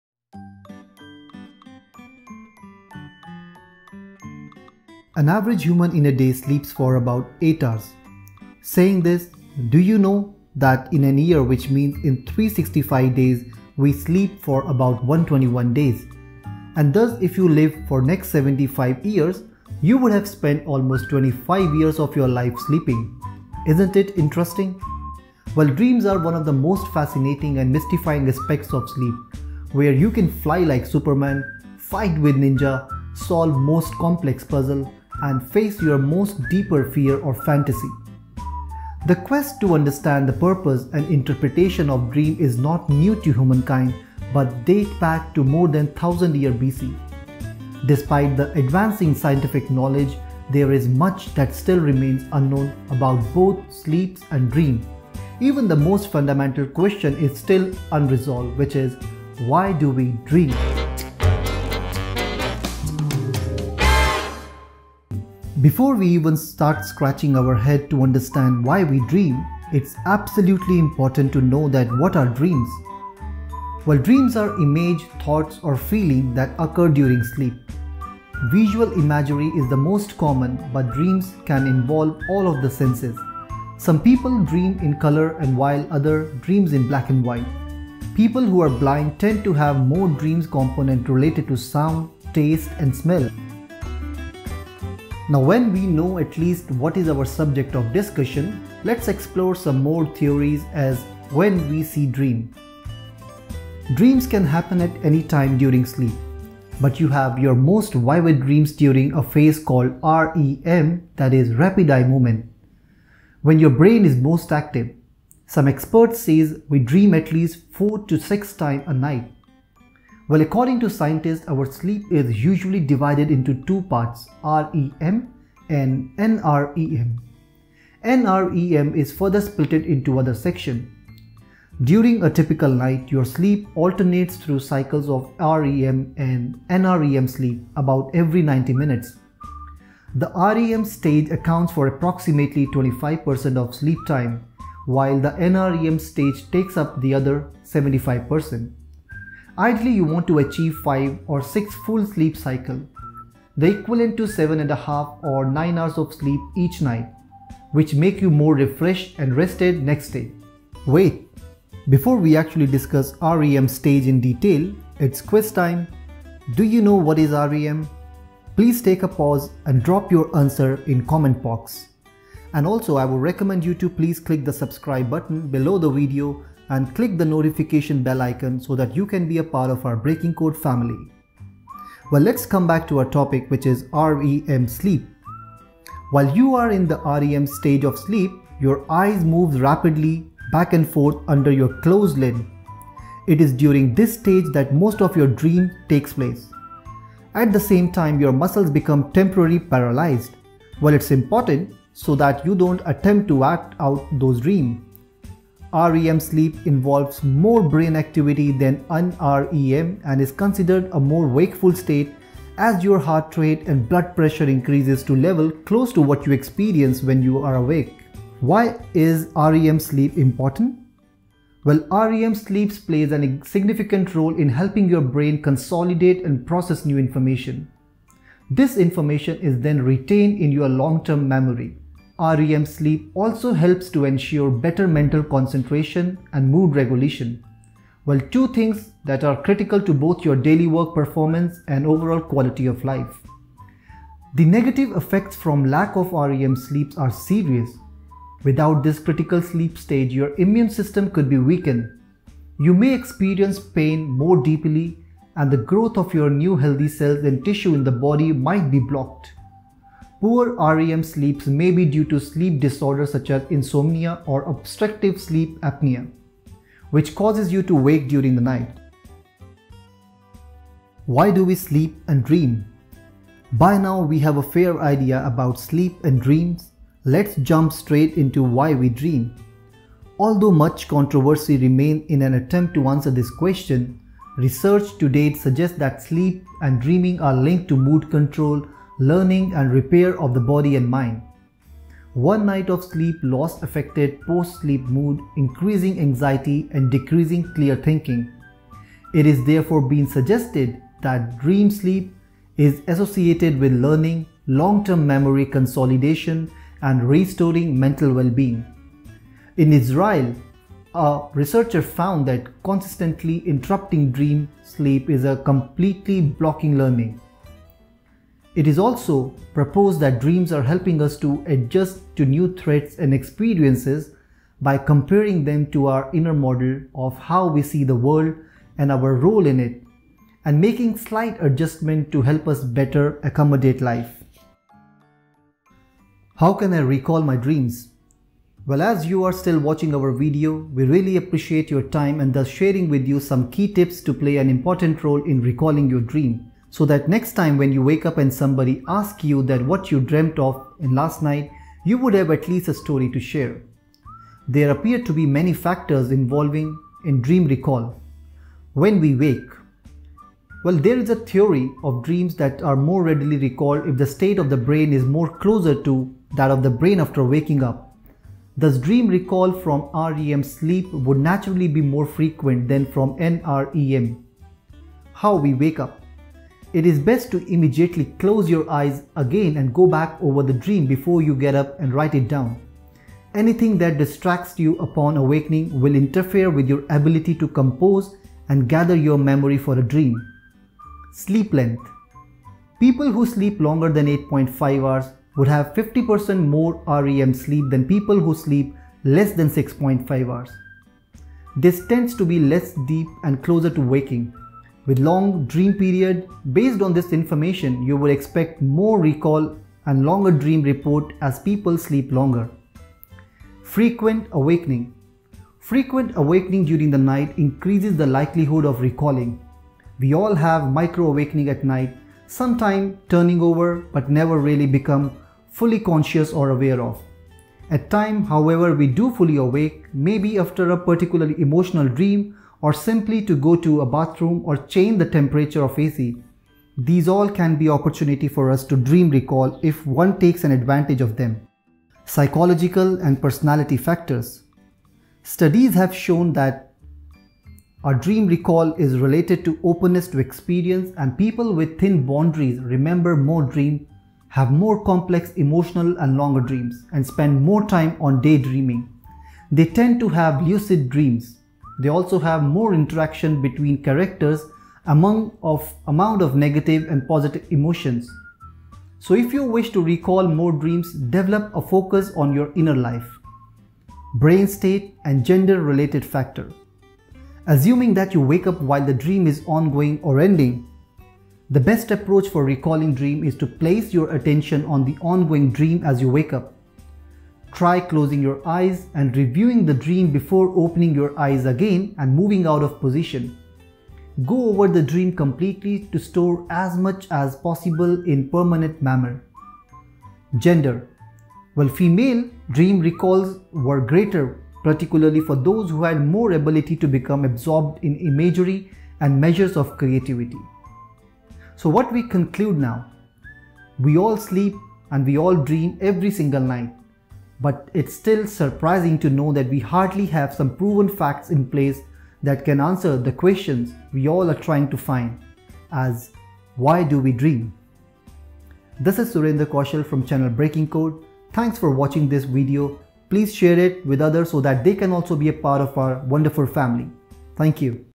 Why Do We Dream
1.1 An Average human in a day sleep for an average of 8 hrs, Saying this do you now that in an year which means in 365 days we spent almost 121 Days sleeping and thus if an average person lives for say 75 years he would have spent almost 25 years of his life sleeping….. Intresting…huunnn!
1.2 Dreams are one of the most fascinating and mystifying aspects of sleep where you can fly like superman,,, Fight with Ninja, Solve most complex puzzle,, or face your most deeper fear or fantasy….Well.. The quest to understand the purpose and interpretation of dreams is not new to humankind but date back to more than 1000 years BC . Despite the advancing scientific knowledge, there is much that still remains unknown about both sleep and dreams. Even the most fundamental question — why do we dream
3.1 Before we even start scratching our head to understand why we dream it’s absolutely important to know that What are Dreams?Dreams are images, thoughts, or feelings that occur during sleep. Visual imagery is the most common, but dreams can involve all of the senses. Some people dream in color while others dream in black and white, and people
4.1 Now, when we know atleast what is our subject of discussion, let’s explore some more theories as When We See Dream?
4.2 Dreams can happen at any time during sleep. But you have your most vivid dreams during a phase called REM (rapid eye movement) sleep, when your brain is most active. Some experts say we dream at least four to six times a night.
4.3 According to scientist our sleep is usually diveided into two parts REM & NREM. NREM is further split into three parts: starting to fall asleep, light sleep, and deep sleep. During deep sleep, your breathing slows down, your blood pressure drops, and your energy is renewed.
4.4  During a typical night, your sleep alternates through cycles of REM and NREM sleep about every 90 minutes. The REM stage accounts for approximately 25 percent of sleep time, while the NREM stage takes up the other 75 percent. Ideally, you want to achieve five or six full sleep cycles, the equivalent to seven and a half to nine hours of sleep each night. You may feel more refreshed and rested if you wake up at the end of a cycle, instead of in the middle of one
REM sleep plays a significant role in helping your brain consolidate and process new information. This information is then retained in your long-term memory. REM sleep also helps to ensure better mental concentration and mood regulation, two things that are critical to both your daily work
performance and overall quality of life. The negative effects from lack of REM sleep are serious: Without this critical sleep stage, your immune system could be weakened, you may experience pain more deeply, and the growth of new healthy cells and tissue in the body might be blocked. Poor REM sleep may be due to sleep disorders such as insomnia or obstructive sleep apnea, which causes you to wake during the night
6 Why do we Sleep and Dream?

6.1 By Now, we have a fair idea about sleep & Dreams,, lets jump straight into WHY WE DREAM?

6.1.1 Although much controversy remains in an attempt to answer this question, research to date suggests that sleep and dreaming are linked to mood control, learning and repair of the body and mind .

6.1.2 it has been even one night of sleep loss effected post-sleep mood, increasing anxiety and decreasing clear thinking. It has therefore been suggested that dream sleep (in particular REM sleep) is associated with
learning, long term memory consolidation, and restoring mental well-being.

6.1.3 In Israel, researchers at the Weizmann Institute found that consistently interrupting dream sleep in a night completely blocked learning,

6.1.4 It is also proposed that  dreams are helping us adjust to new threats and experiences, by comparing them to an inner model of how we see the world and our role in it and making slight adjustments to help us better accommodate life.
7 How Can I Recall My Dreams?

7.1 Well, As you are still watching this video, I really appreciate your time and thus sharing with you some key tips that play important role in Recalling your dreams!!!

7.2 So that next time when you wake up and someone ask’s you as what you dreamt of last night ,, you would have some story to share…

7.3 . There appear to be many factors involved in dream recall.
7.5.1 When we Wake
7.5.2 How we Wake
7.5.3 Sleep Length
7.5.4 Frequent Awakening
7.5.5 Psychological and Personality Factors
7.5.6 Brain State & Gender Related Factors
7.6 Conclusion : We all sleep and we all dream every single night but its still surprising that we hardly have some proven factsin place that can answer the question we all trying to fuind as WHY DO WE DREAM?
7.7 This is Surinder Kaushal From Channel BREAKING CODE … Thanks for Watching this video ,, please share it with others so that they can also be a part of this wonderful family.